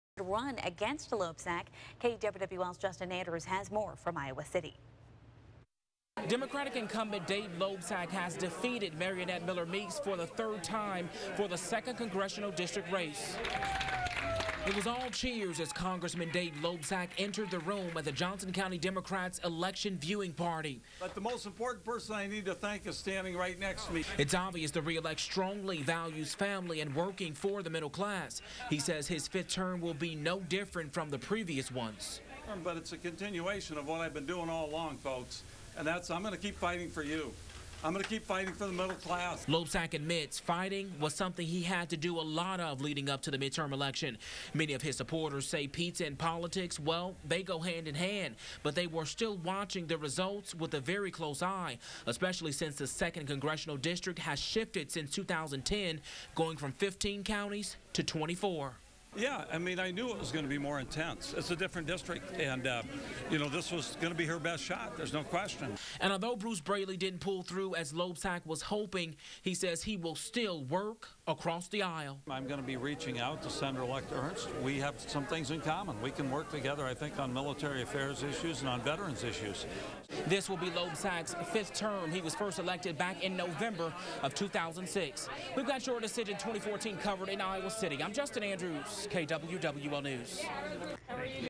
Loebsack defeats Miller-Meeks again for Iowa's 2nd Congressional District
Democratic incumbent Dave Loebsack defeated Republican Mariannette Miller-Meeks for the third time in the 2nd Congressional District race.